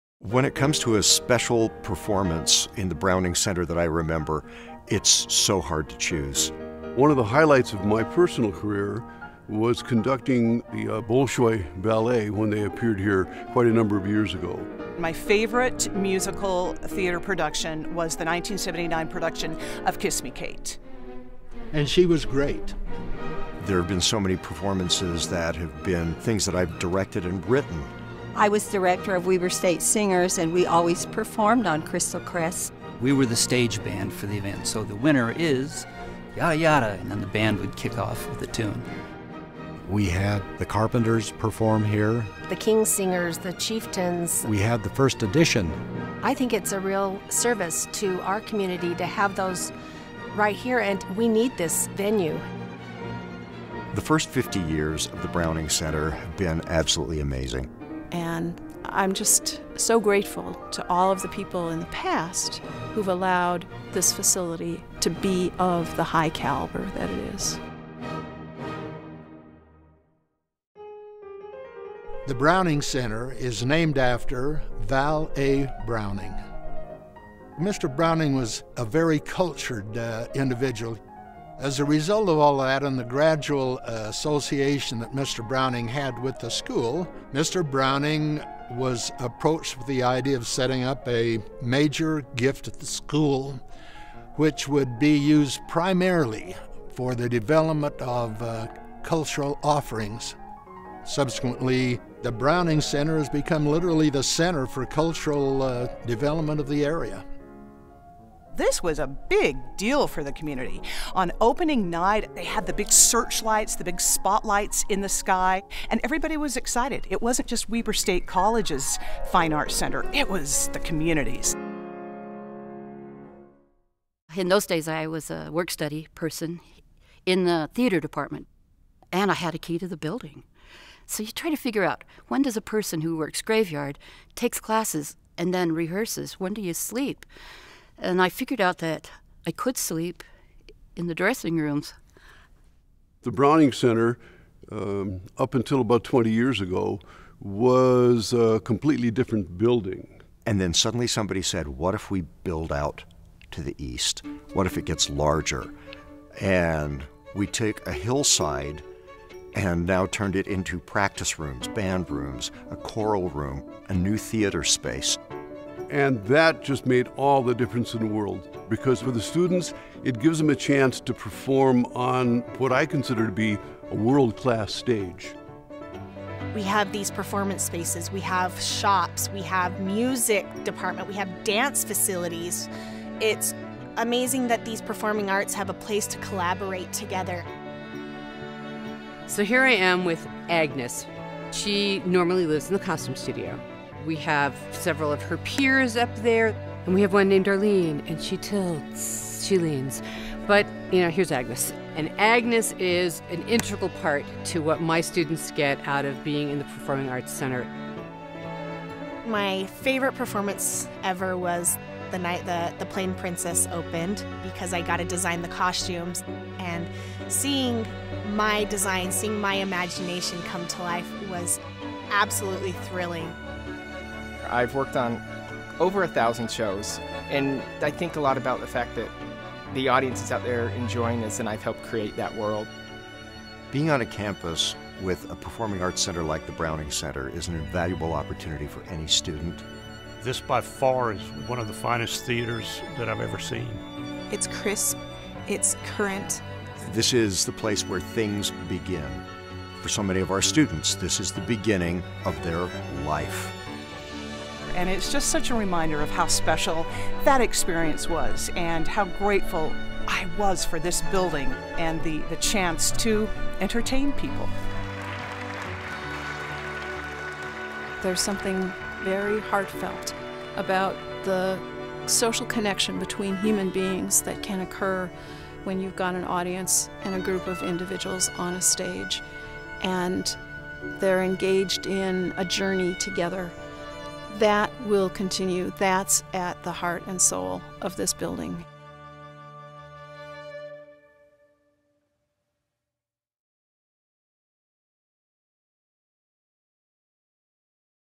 The History of the Val. A Browning Center at Weber State University
The film shares memorable stories and events of the Val A. Browning Center, Weber State University's performance hall, for over 50 years.